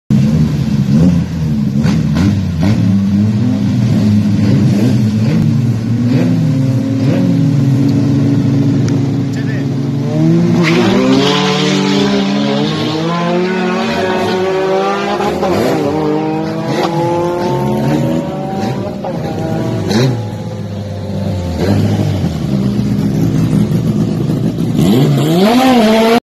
Chevrolet Celta MauBonRacing vs Corsa Swap 2.4 Wilde Clase 5 (10.15)
Celta 10.25 con 00.06 de reaccion / Corsa 10.39 con 00.25 de reaccion 250 metros a radiales spider